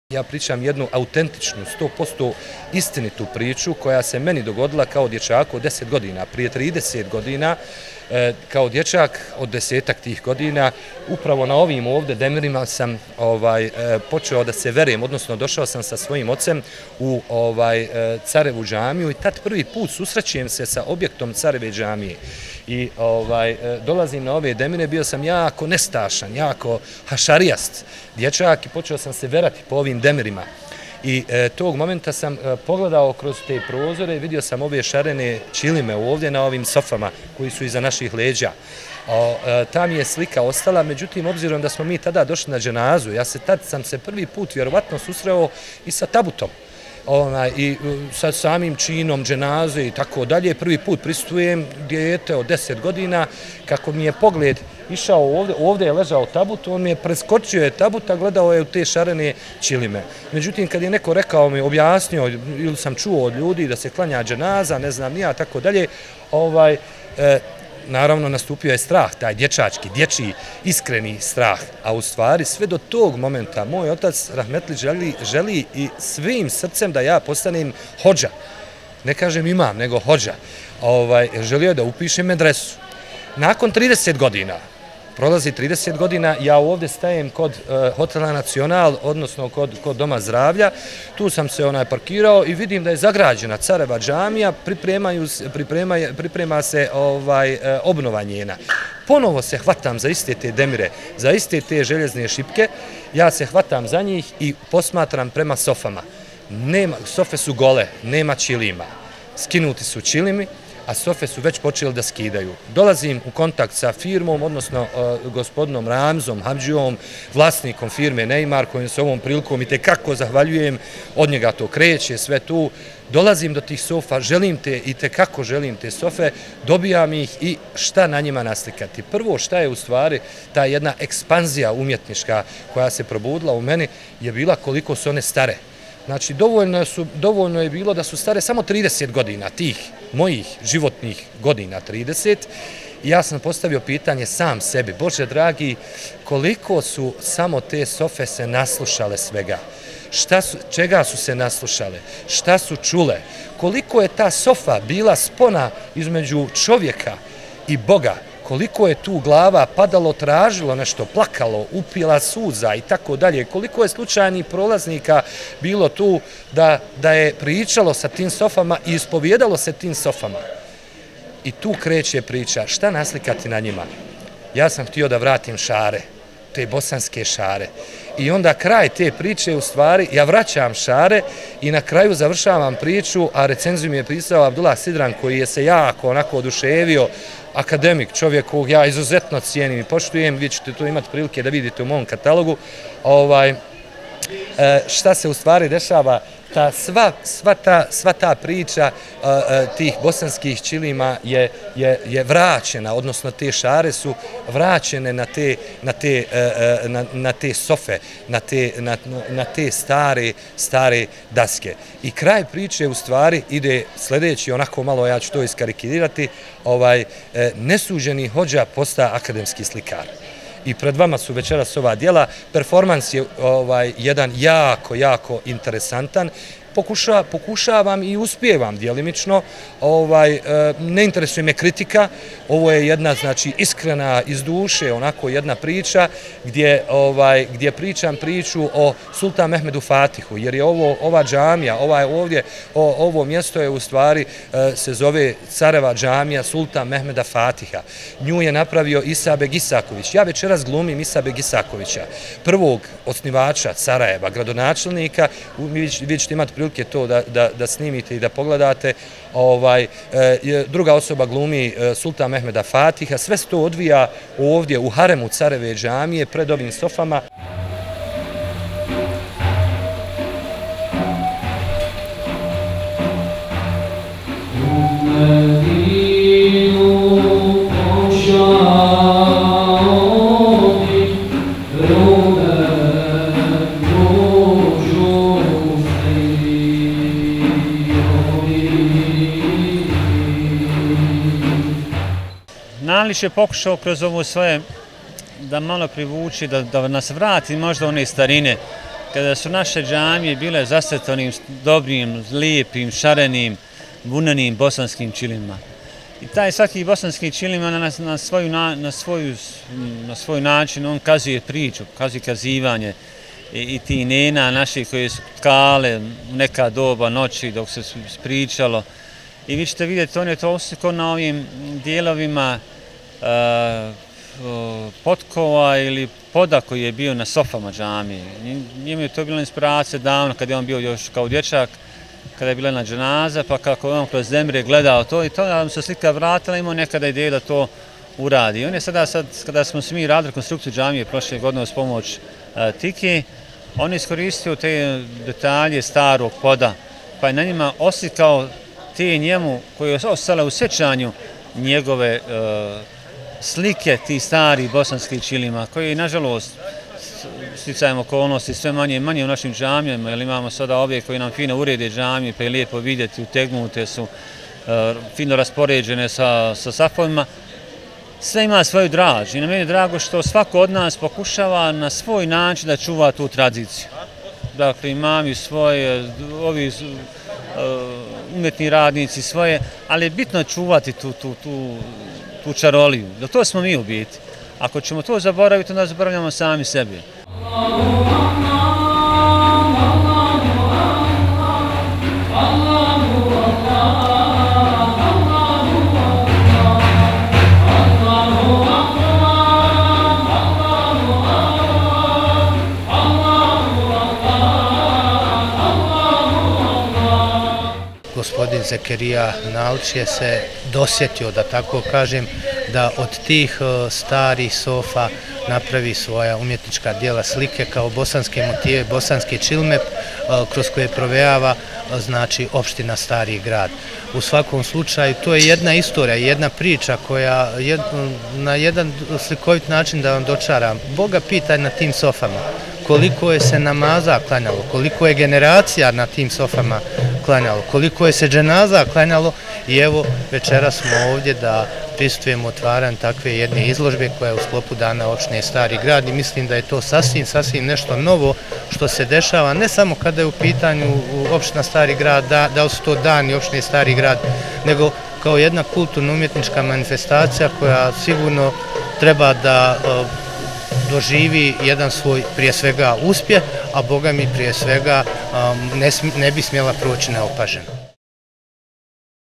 Sarajevske sultanove sofe
U haremu Careve džamije otvorena je izložba "Sarajevske sultanove sofe", akademskog slikara Zekerijaha Nalića.

Da se pretplatite na Hema TV Youtube Kanal click na link ispod